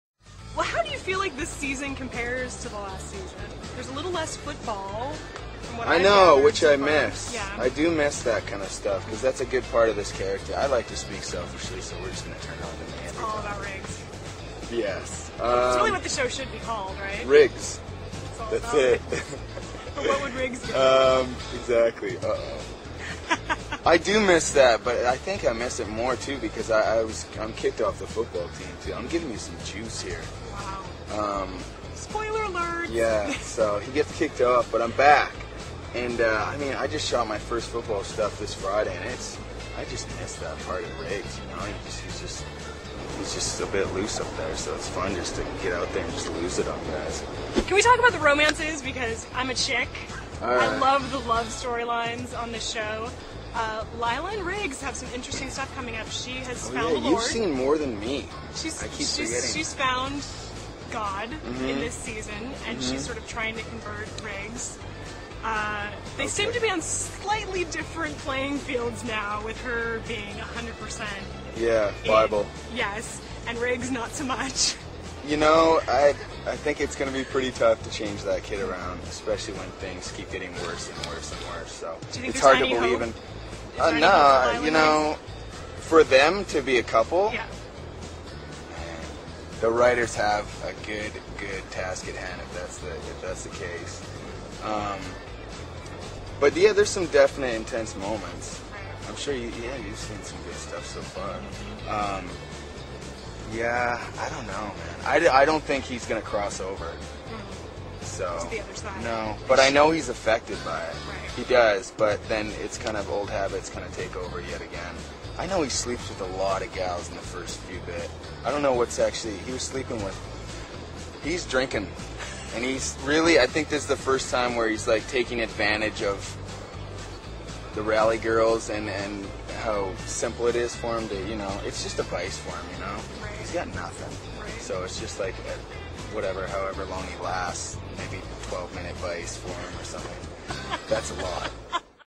Taylor Kitsch talks about his character Tim Riggins
Taylor Kitsch discusses season 2 and his character Tim Riggins on the TV show you should be watching Friday Night Lights.